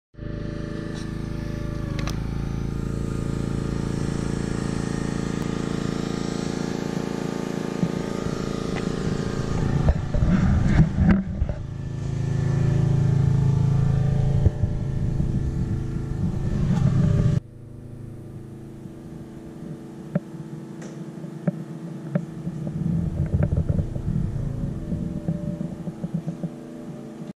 Looking inside a (NEW) septic tank!
Ever wonder what the inside of a septic tank looks like? I did  too. Check it out!

What's inside a septic tank? What does a septic tank look like?